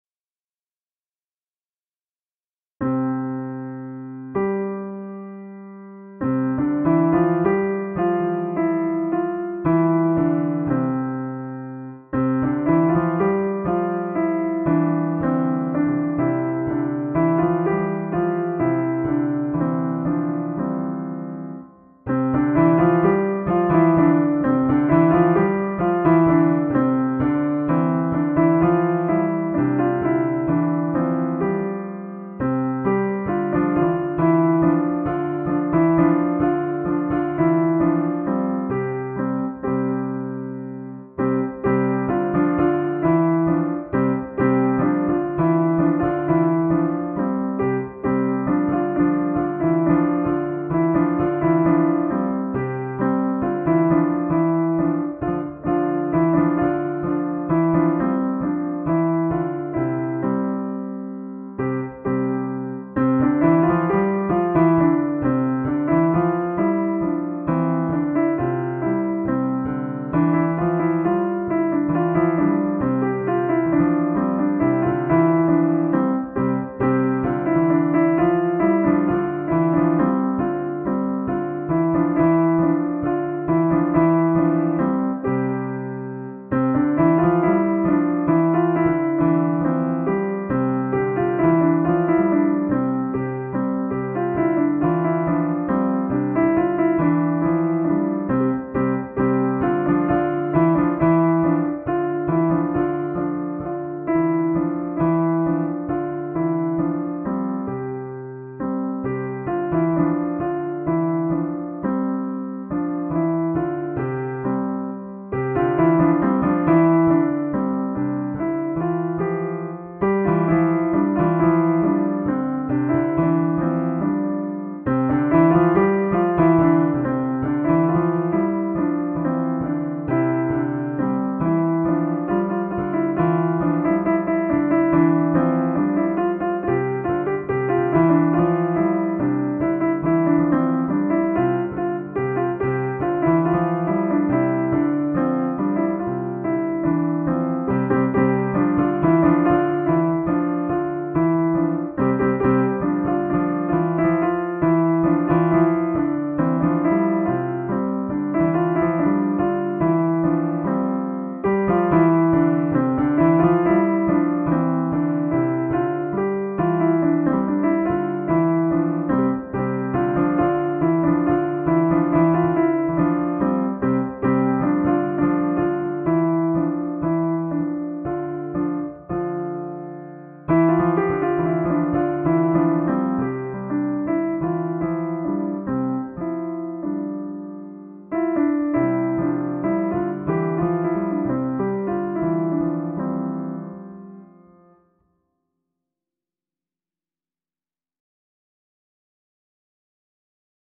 The vase with white flowers
The musical investments of the photos are all improvisations.